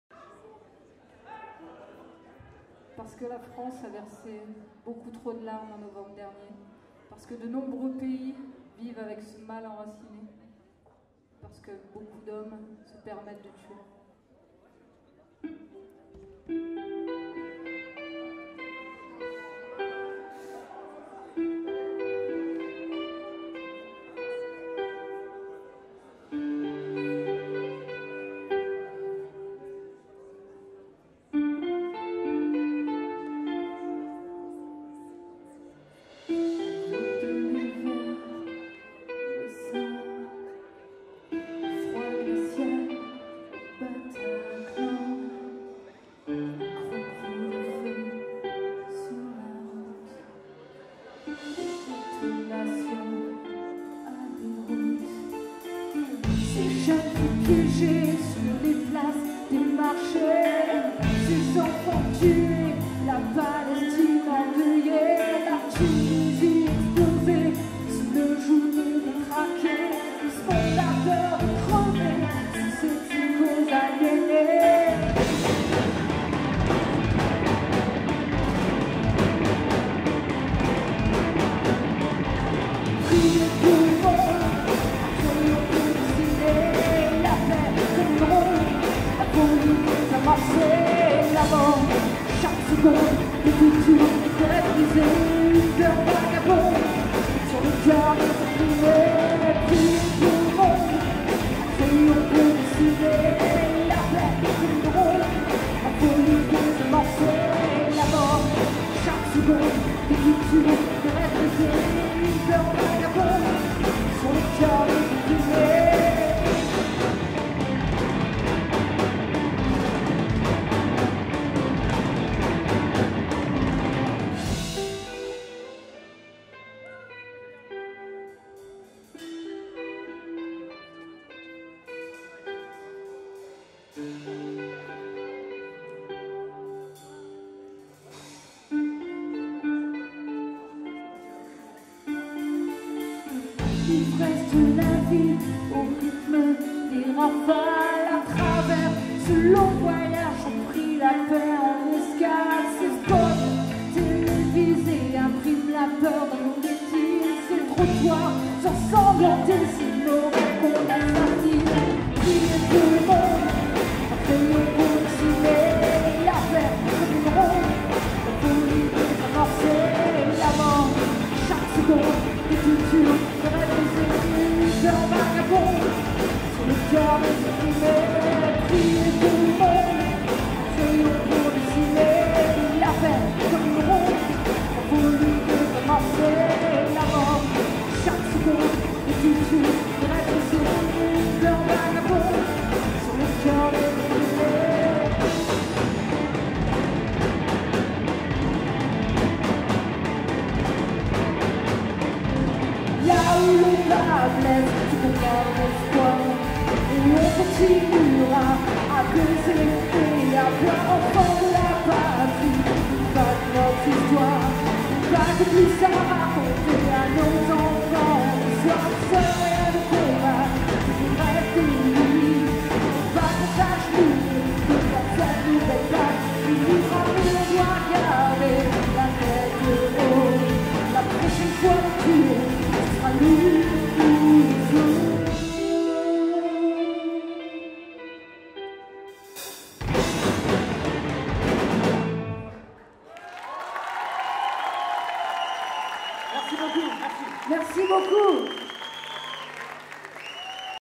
Finale Jukebox 2016 : CHEEKY
Le groupe Cheeky, live sur la scène du Nouveau Relax à Chaumont, le vendredi 3 juin 2016. Finale du tremplin Jukebox édition 6, organisé par le Journal de la Haute-Marne.